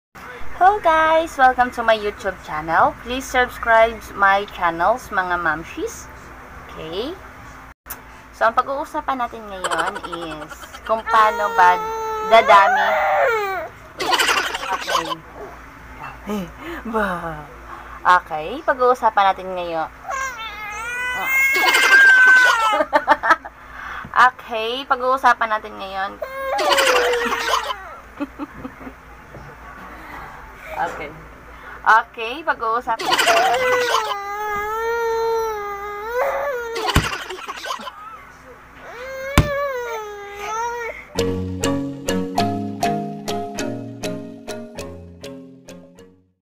Baby'ng pa cute
Gustong mag pa cute.  Gustong NASA harap sya Ng camera. 😂🤣 Pabida baby ko 🤣😍😘